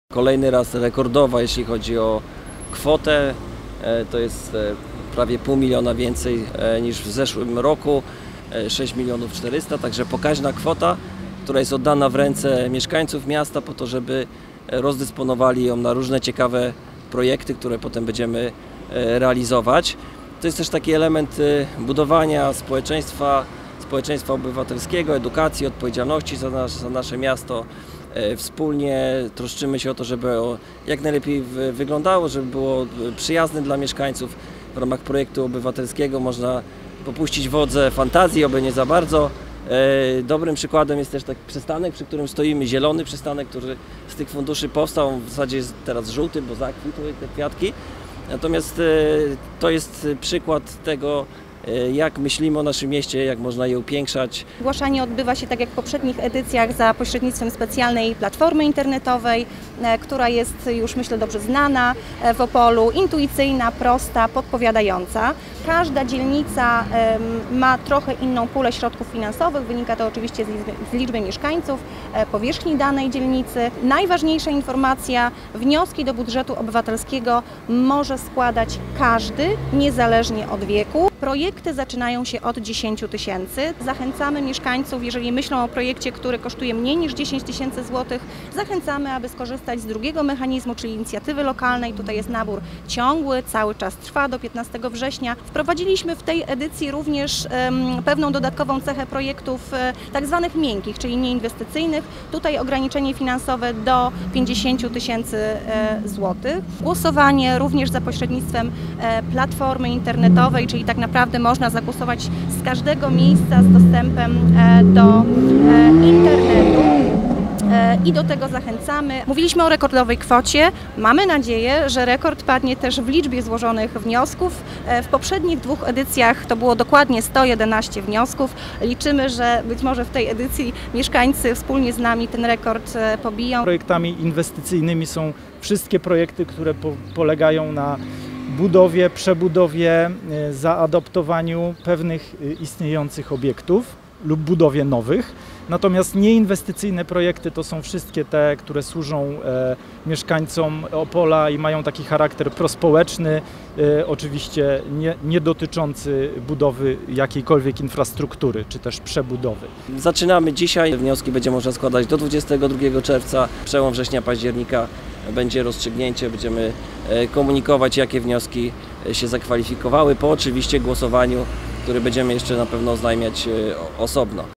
Rusza 8. edycja Budżetu Obywatelskiego Opola
Od dziś możecie zgłaszać swoje projekty do Budżet Obywatelski Opola.